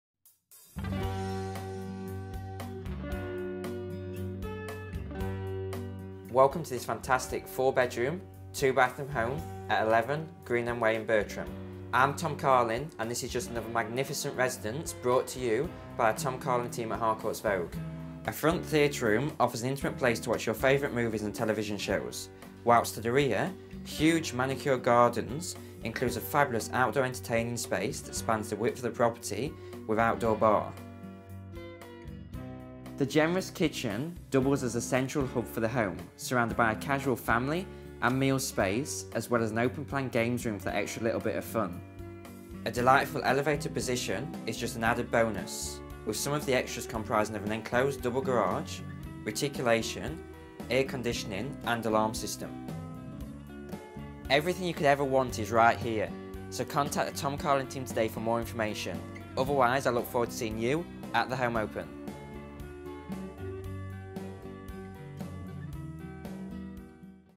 11 Greenham Way, Bertram by Tom Carlin
Welcome to this fantastic four-bedroom two-bathroom home at eleven (11) Greenham Way in Bertram &ndash; I&rsquo;m Tom Carlin and this is just another magnificent residence brought to you by my team at Harcourts Vogue.
The generous kitchen doubles as the central hub of the house, surrounded by a casual family and meals space as well as an open games area for that little extra bit of fun.
A front theatre room offers an intimate place to watch your favourite movies and television shows, whilst to the rear, a huge garden includes a fabulous outdoor entertaining patio that spans the entire width of the property.
A delightful elevated position is just an added bonus with some of the extras comprising of an enclosed double garage and reticulation.
Everything you need and more is right here &ndash; so contact the Tom Carlin Team today for more information or to organise a viewing.